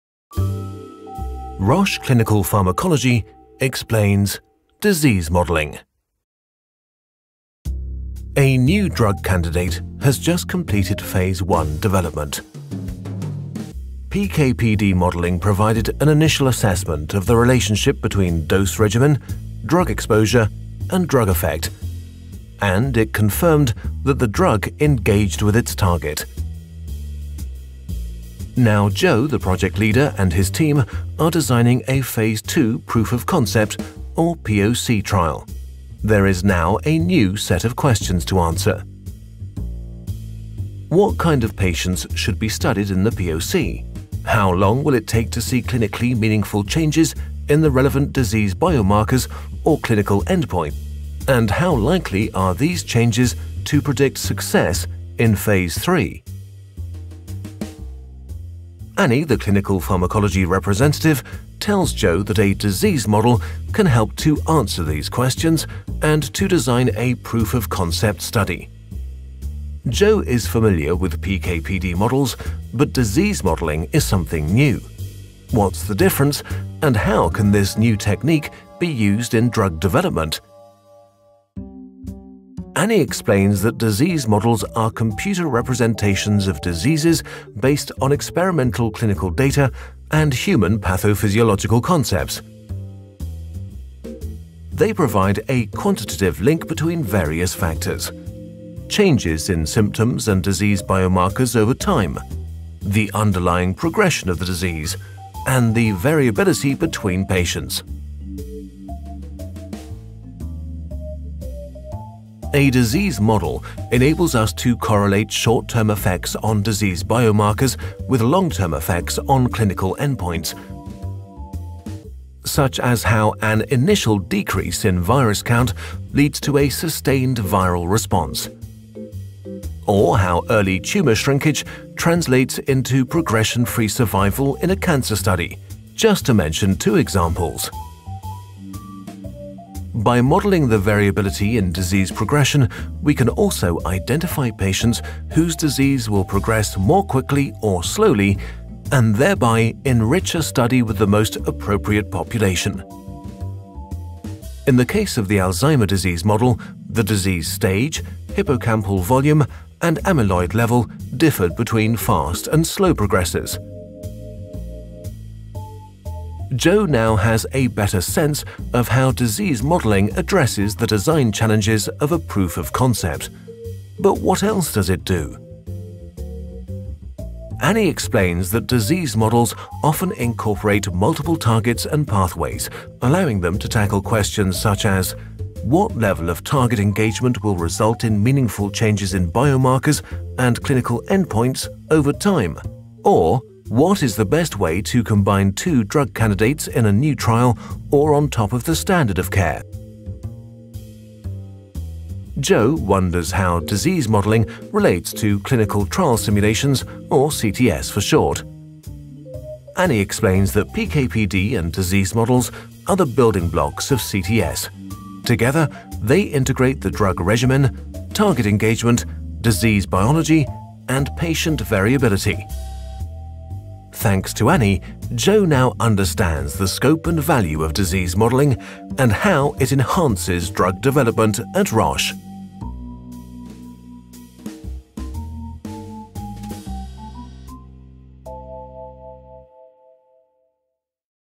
Disease modeling to inform clinical trial simulation
Roche pRED Clinical Pharmacology invested in disease modeling capabilities to impove Clinical Trial Simulation predictive performance. Disease models are computer representations of diseases, based on experimental clinical data and human pathophysiological concepts. They provide quantitative links between changes in symptoms and disease biomarkers over time, the underlying progression of the disease and the variability between patients.